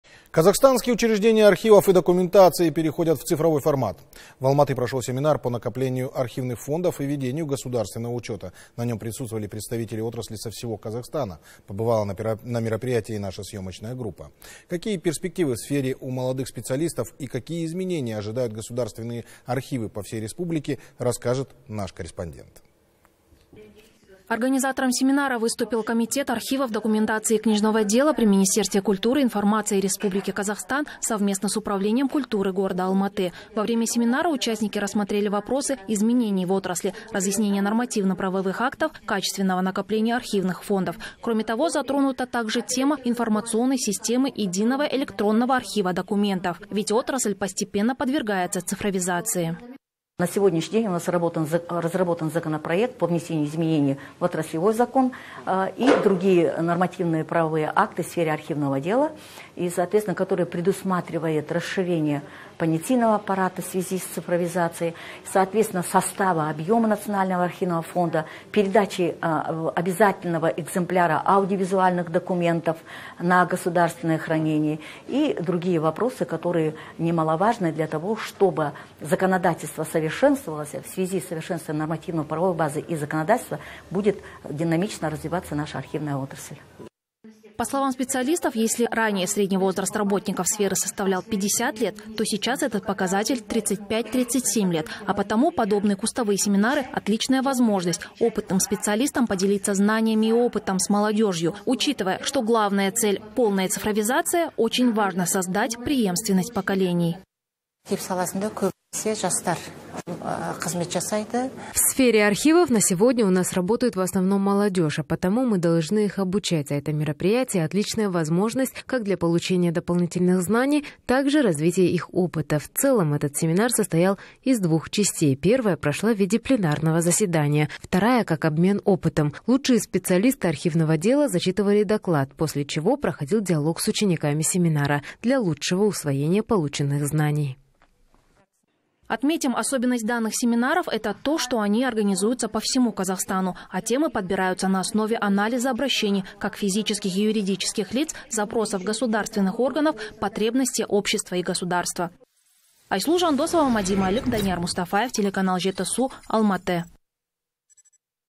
Казахстанские учреждения архивов и документации переходят в цифровой формат
Казахстанские учреждения архивов и документации переходят в цифровой формат. В Алматы прошел семинар по накоплению архивных фондов и ведению государственного учета. На нем присутствовали представители отрасли со всего Казахстана, побывала на мероприятии и наша съемочная группа